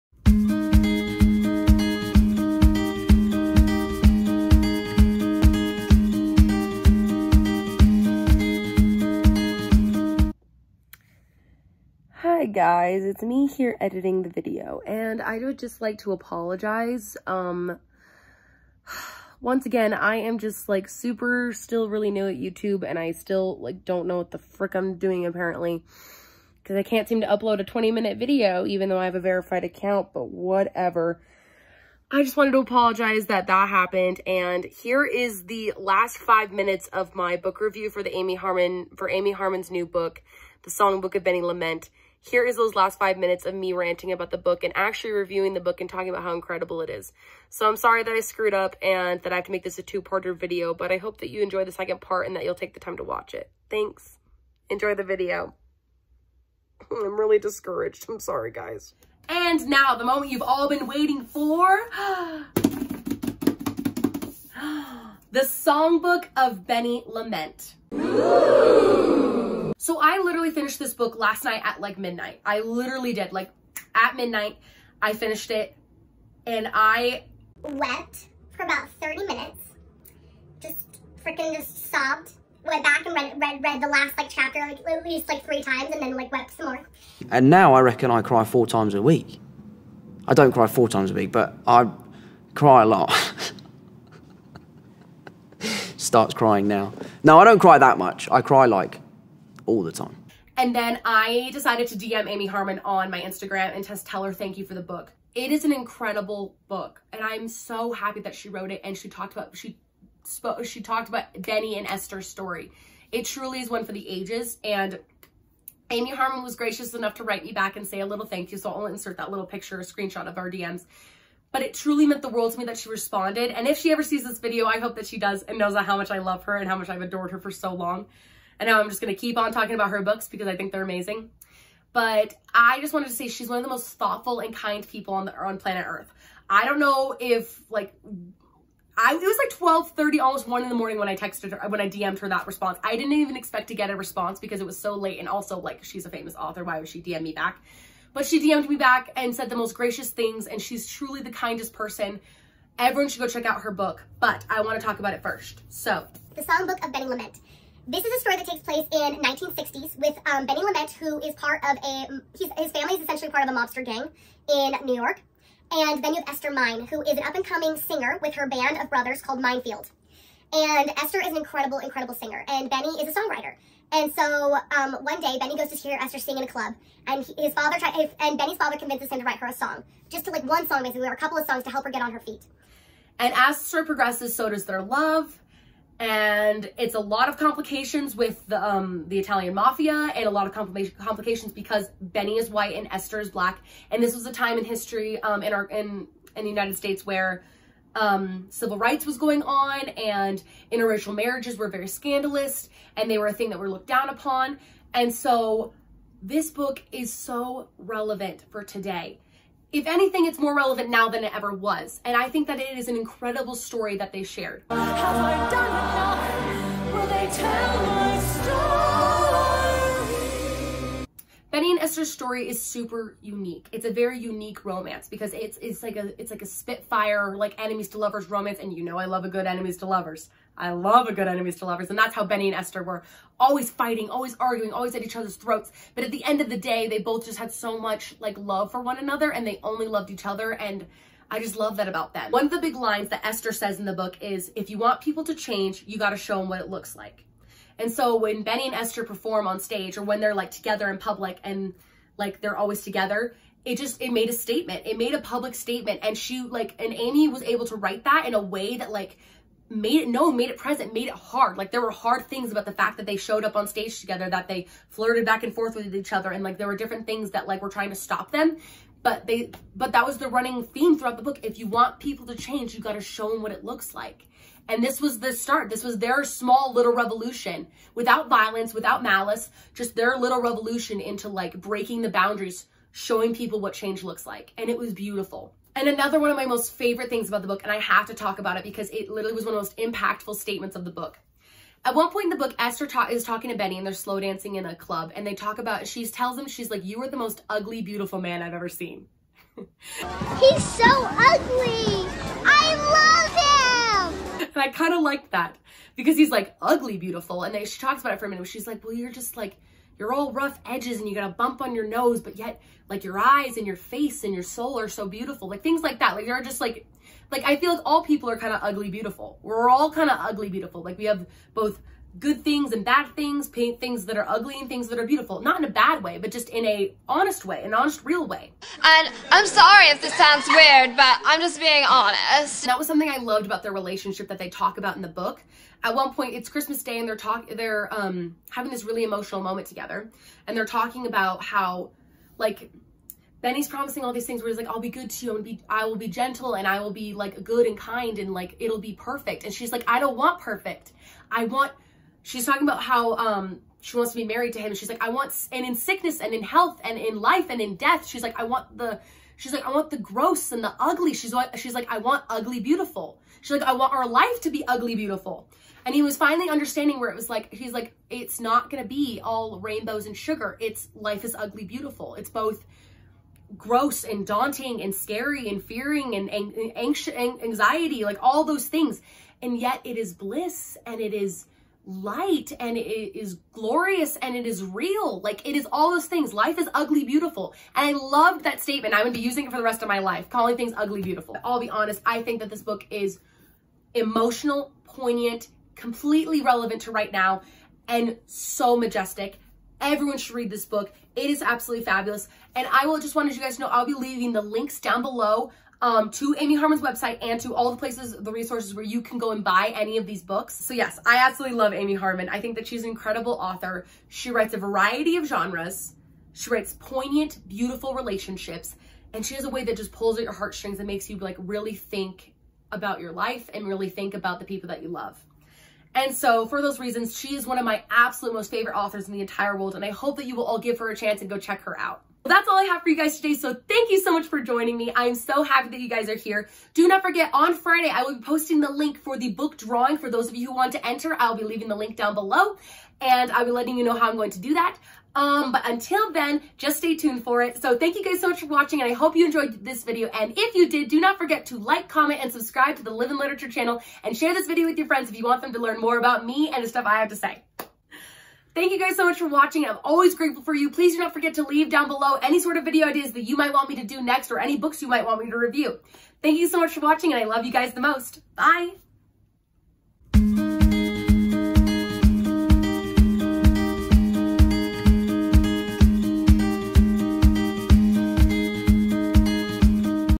Favorite Author Tag: Amy Harmon Part 2
Hey guys! This is part two of my Favorite Author Tag! In today'a video I am talking about Amy Harmon and why I love her! And I will be talking about all her incredible books, especially her latest novel "The Songbook of Benny Lament." This book like her many other books is absolutely incredible and everyone should read it! Be on the look out in my next video for a link to enter the 100 Subscriber Book Drawing!!! Thanks so much for everything you guys are the best!

The Songbook of Benny Lament
Where the Lost Wander
Making Faces
A Different Blue
The Bird and the Sword